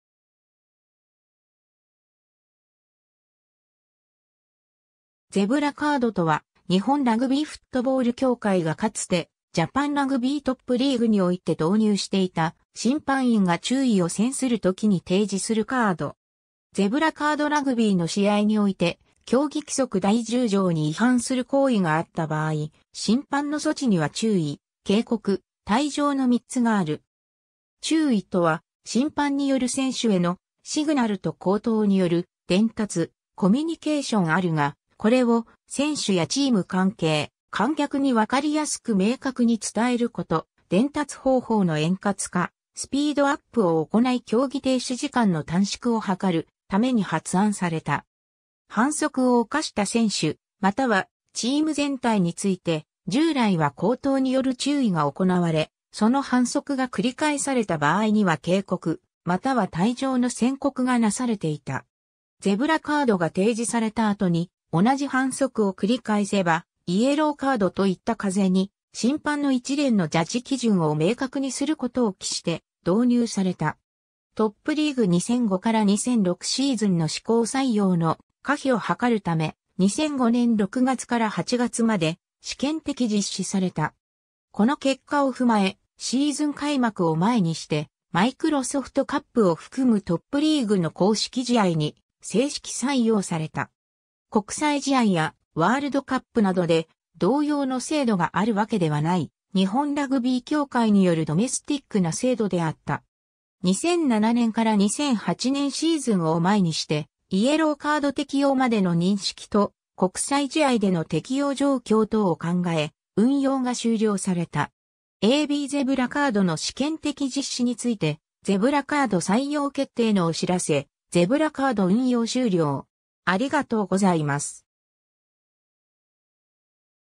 ゼブラカード
ゼブラカード（Zebra Card）とは、日本ラグビーフットボール協会がかつてジャパンラグビートップリーグにおいて導入していた、審判員が注意を宣するときに提示するカード。
 ゼブラカード ラグビーの試合において、競技規則第10条（不正なプレー）に違反する行為があった場合、審判の措置には注意・警告（イエローカード、シンビン）・退場（レッドカード）の3つがある。
注意とは審判による選手への「シグナルと口頭による伝達・コミュニケーション」あるが、これを選手やチーム関係、観客にわかりやすく明確に伝えること、伝達方法の円滑化・スピードアップを行い競技停止時間の短縮を図るために発案された。
 反則を犯した選手またはチーム全体について、従来は口頭による注意が行われ、その反則が繰り返された場合には警告または退場の宣告がなされていた。
「ゼブラカードが提示された後に同じ反則を繰り返せばイエローカード」といった風に、審判の一連のジャッジ基準を明確にすることを期して導入された。
 トップリーグ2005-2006シーズンの施行採用の可否を諮るため、2005年6月から8月まで試験的実施された。
この結果を踏まえ、シーズン開幕を前にしてマイクロソフトカップを含むトップリーグの公式試合に正式採用された。
 国際試合やワールドカップなどで同様の制度があるわけではない、日本ラグビー協会によるドメスティックな制度であった。
2007年-2008年シーズンを前にして、イエローカード適用までの認識と、国際試合での適用状況等を考え、運用が終了された。
 ^ a b ゼブラカード(仮称)の試験的実施について ^ 「ゼブラ・カード」採用決定のお知らせ ^ 「ゼブラカード」運用終了